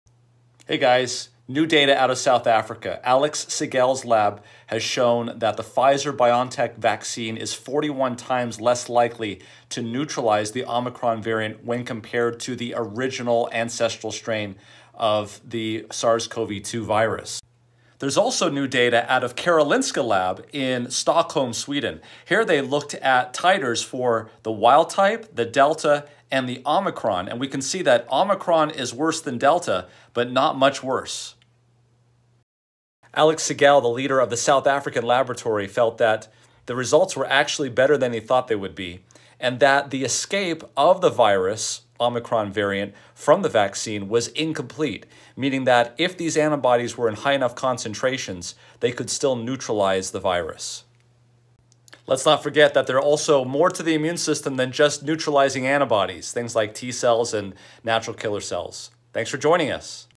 Omicron data on neutralizing antibodies
New data out of South Africa and Sweden on the ability of the Pfizer vaccine to neutralize the Omicron variant.